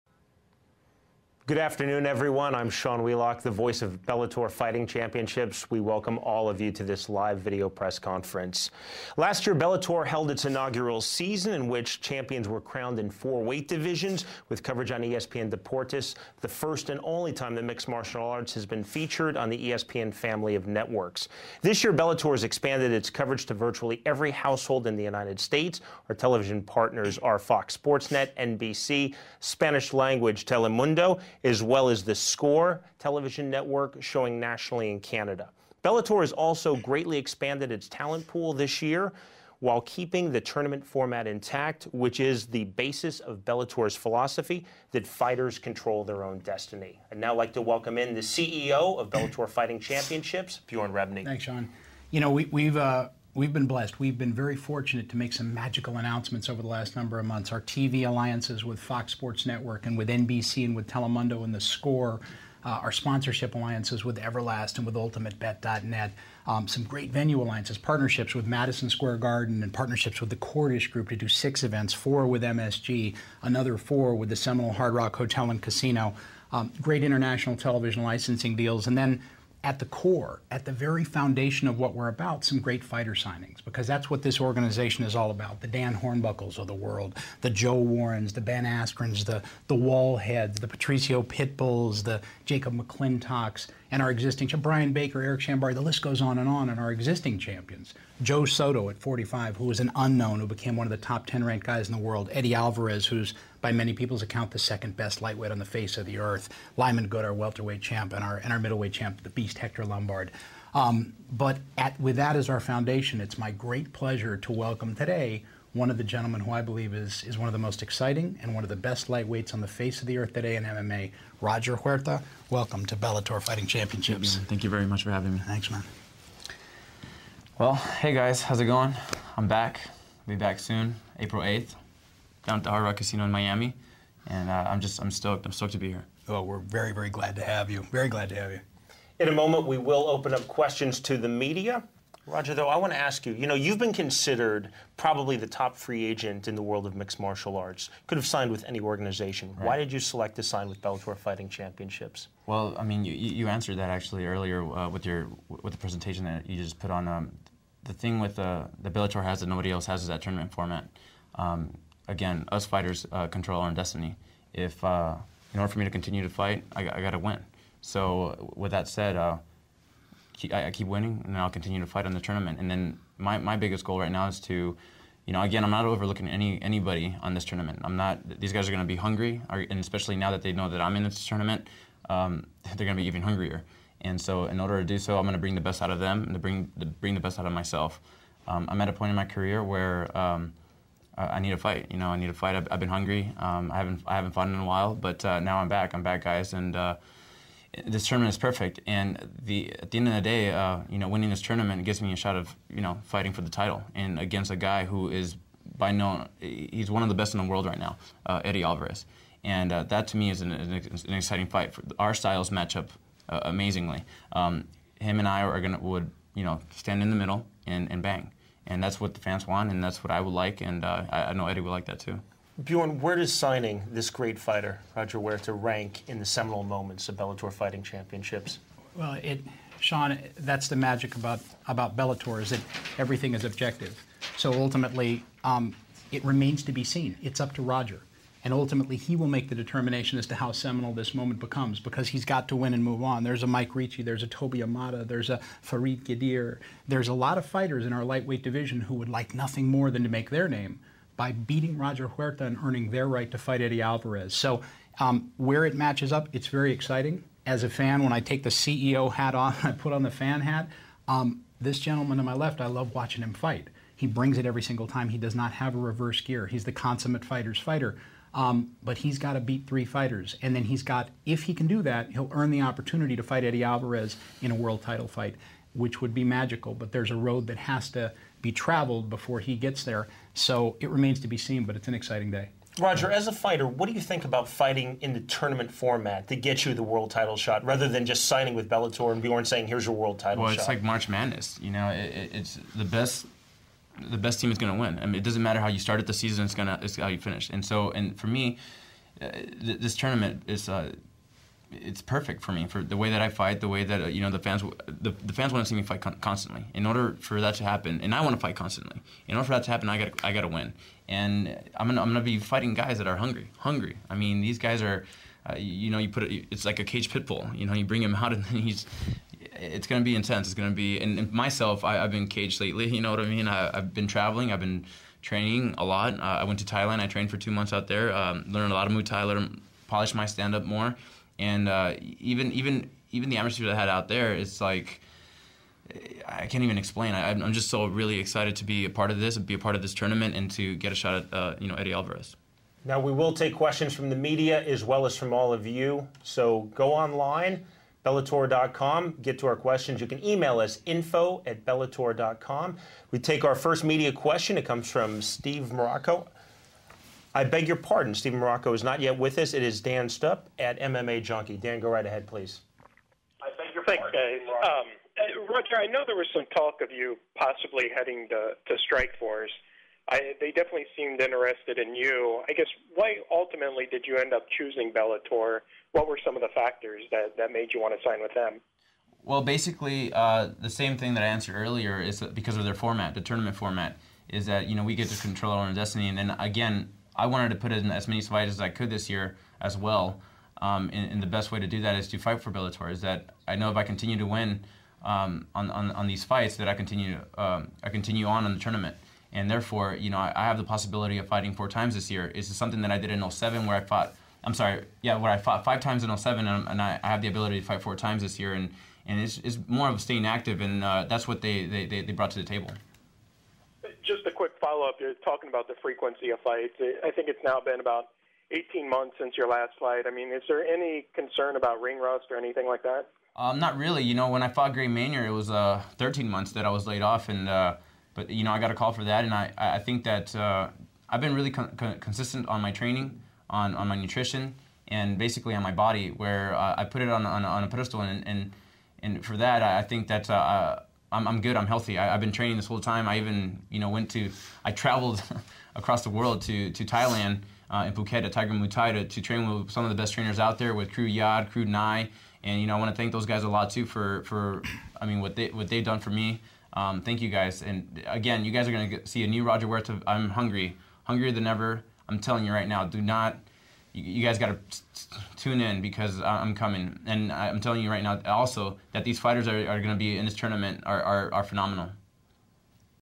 Bellator MMA: Roger Huerta Press Conference
Roger Huerta, the top free agent in MMA, announced on 3/15/10 that he has signed with Bellator to compete in the upcoming Season 2 lightweight tournament.